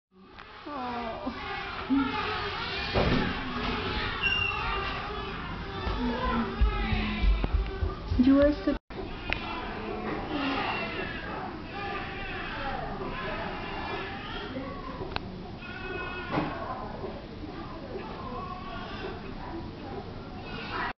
Eastern Social Welfare Society in Korea | Maternity Home
Babies waiting to be adopted from ESWS in South Korea (August 2008)